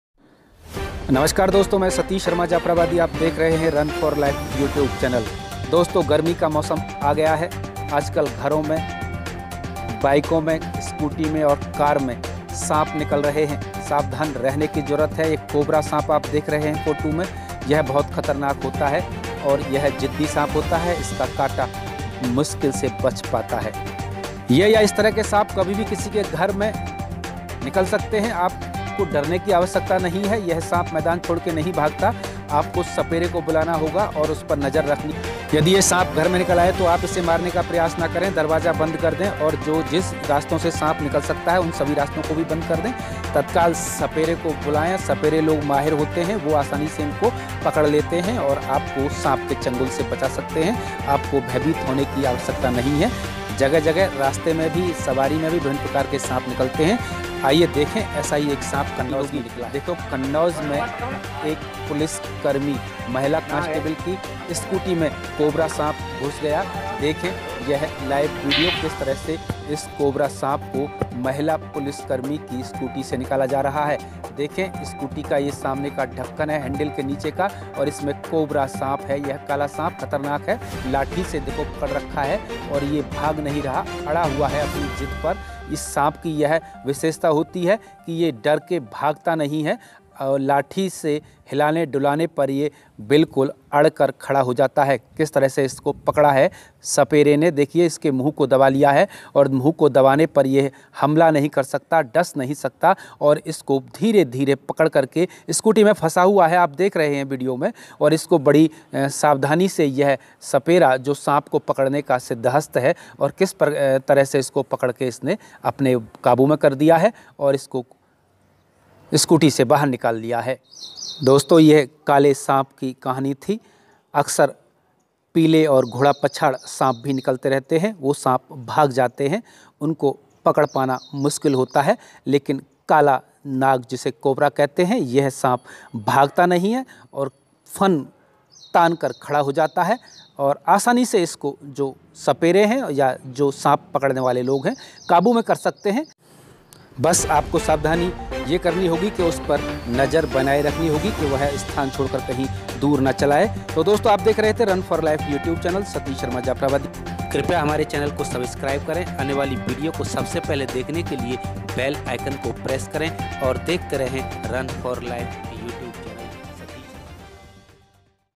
घर वाहन में काला नाग तो कैसे बचेंrun for life#satish sharma#skooti me black sneck#
black snack#
scooti me snack#
pulice station sneck#
स्कूटी कार और घर से कैसे निकलवाए काला जहरीला जानलेवा सर्प देखें पूरा वीडियो